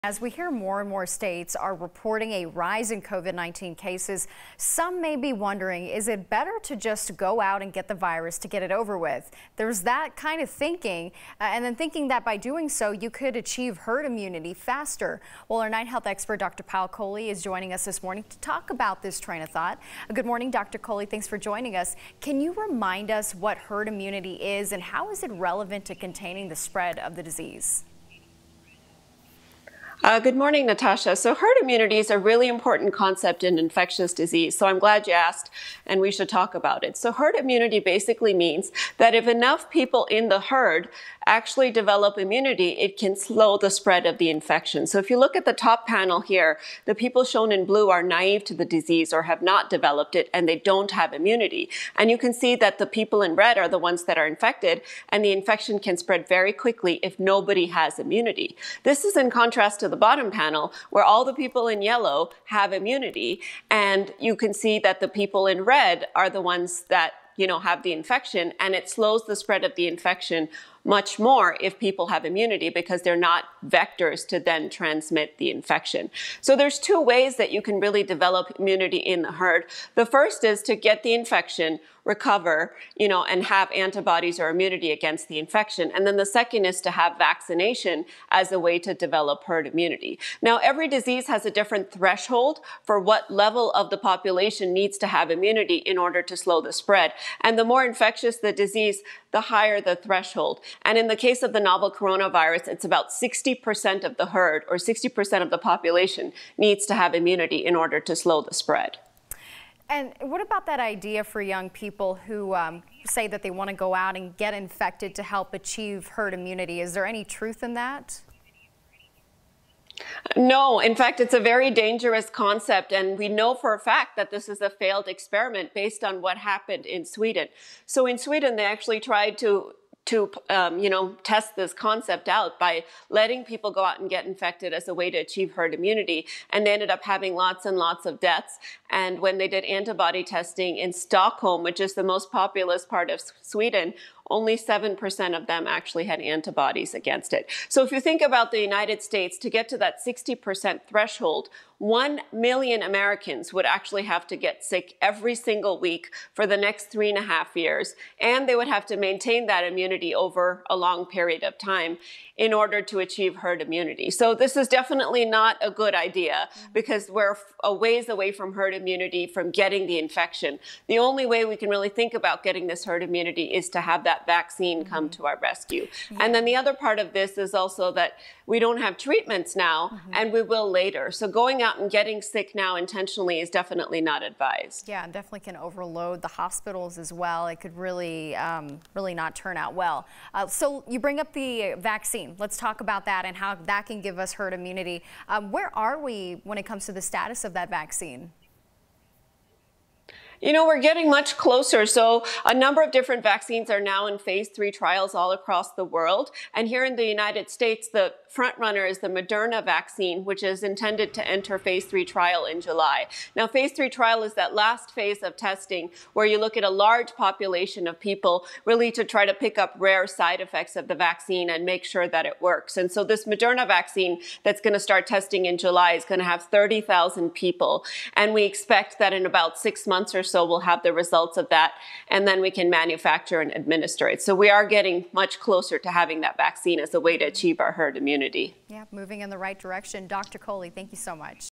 Herd immunity for COVID-19 reexplored
Is it better to just go out and try to get COVID-19 to achieve herd immunity? 9Health Expert Dr. Payal Kohli talks about why that's not a good idea.

9NEWS (KUSA) is located in Denver, Colorado.